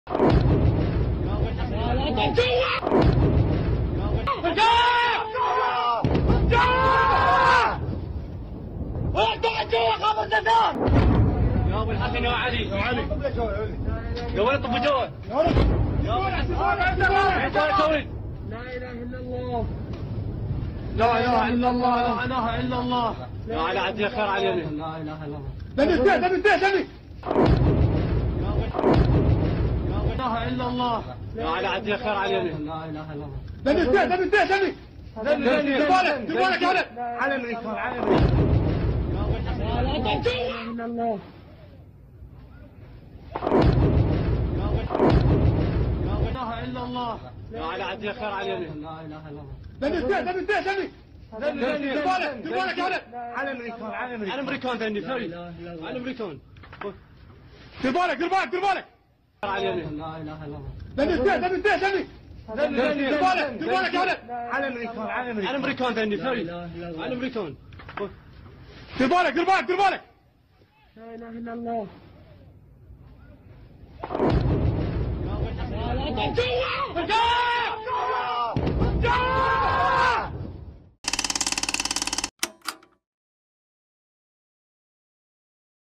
Iran Missile Attack on US Forces: Iran ने किया अमेरिकी सेना पर हमला, देखें वीडियो | KADAK
ईरान (Iran) ने अमेरिका (America) कई मिसाइलें दागने के बाद चेतावनी देते हुए कहा कि अभी तो यह सिर्फ शुरूआत है.

वीडियो सौजन्य: ANI